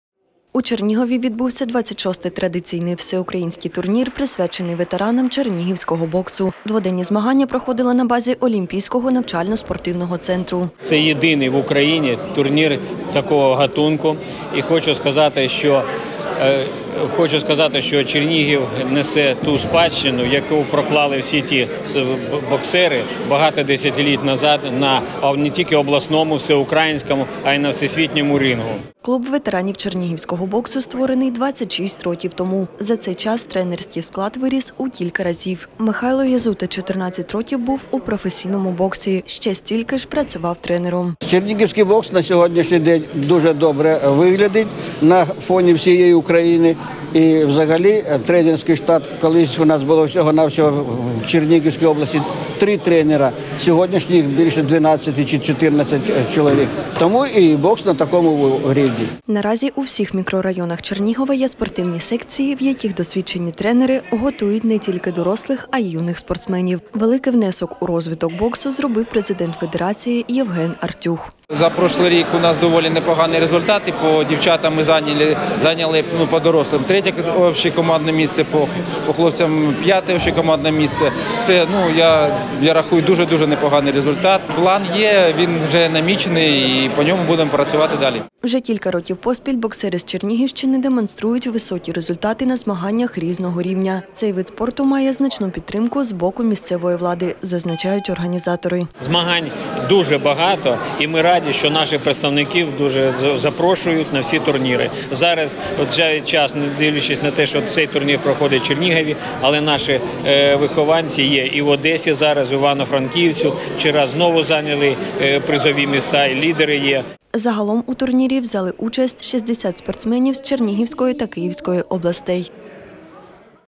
Всеукраїнський турнір з боксу відбувся у Чернігові
У Чернігові відбувся 26-й традиційний всеукраїнський турнір, присвячений ветеранам чернігівського боксу. Дводенні змагання проходили на базі Олімпійського навчально-спортивного центру.
Клуб ветеранів чернігівського боксу створений 26 років тому. За цей час тренерський склад виріс в кілька разів. Михайло Єзута 14 років був у професійному боксі, ще стільки ж працював тренером.
Наразі в усіх мікрорайонах Чернігова є спортивні секції, в яких досвідчені тренери готують не тільки дорослих, а й юних спортсменів. Великий внесок у розвиток боксу зробив президент федерації боксу Євген Артюх.
Вже кілька років поспіль боксери з Чернігівщини демонструють високі результати на змаганнях різного рівня. Цей вид спорту має значну підтримку з боку місцевої влади, зазначають організатори.
Загалом у турнірі взяли участь 60 спортсменів з Чернігівської та Київської областей.